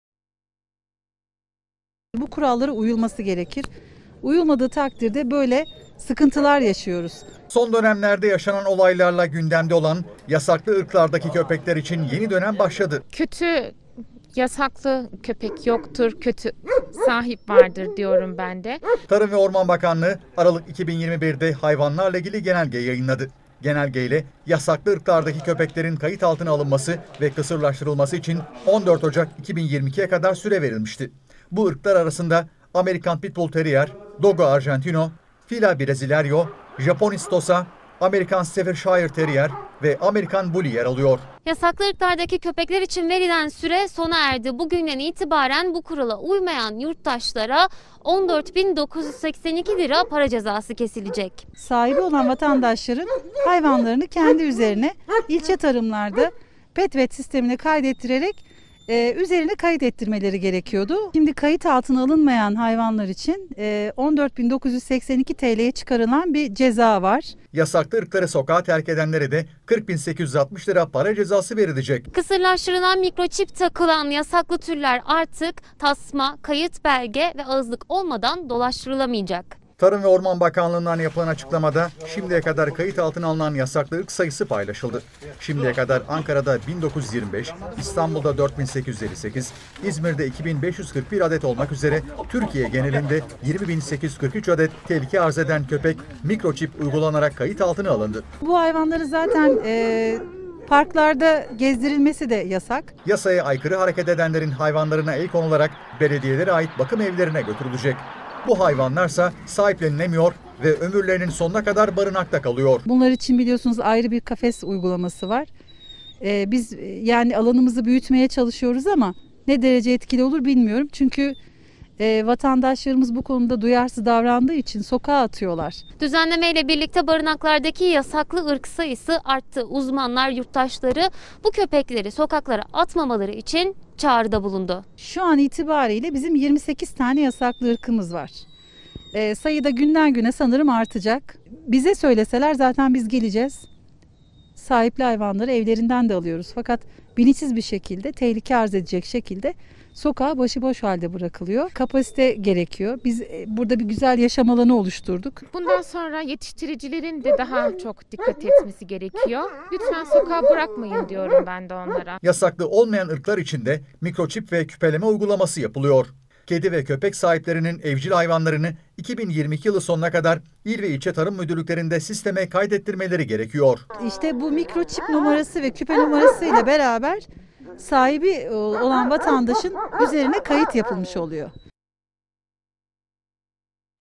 Bakanlık genelge yayımladı: Tehlike arz eden köpek kayıt altına alındı
Tarım ve Orman Bakanlığı, "Bugün saat 16.30 itibarıyla Türkiye genelinde 20 bin 843 adet tehlike arz eden köpek mikroçip uygulanarak kayıt altına alınmıştır" açıklamasını yaptı.